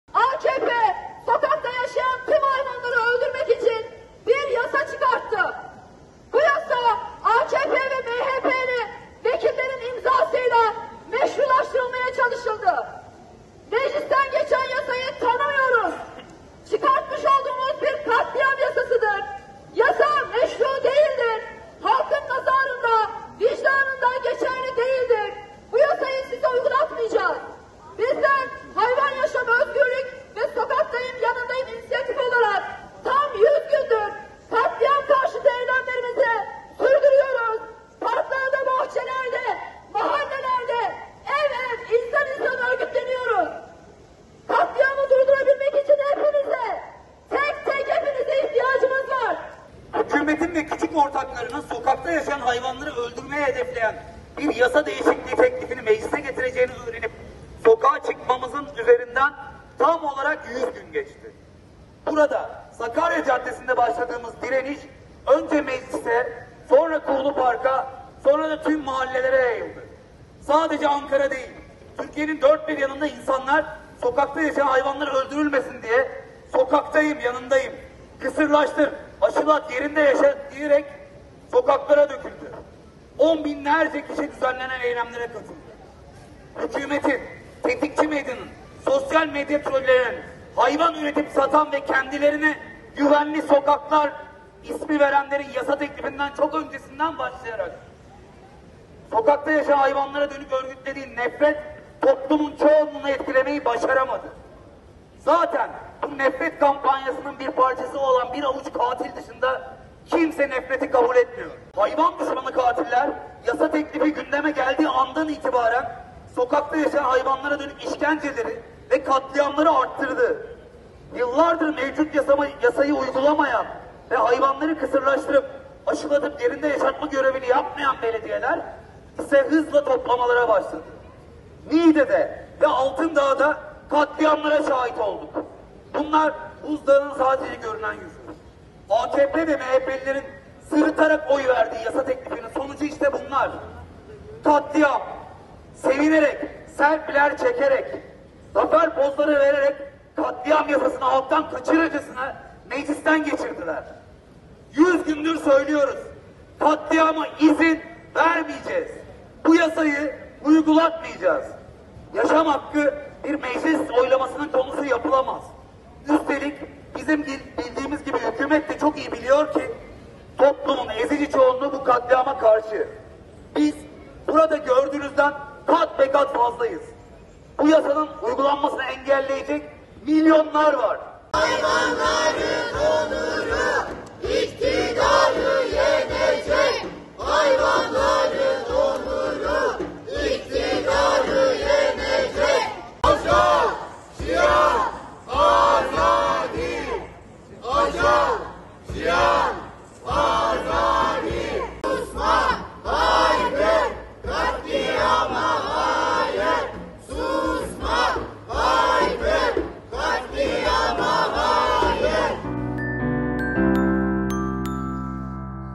YAŞAM NÖBETİ 100’ÜNCÜ GÜNÜNDE
Hayvanların katledilmesinin önünü açan yasanın geri çekilmesi talebiyle yapılan eylemin 100’üncü gününde mücadeleyi büyütme vurgusu.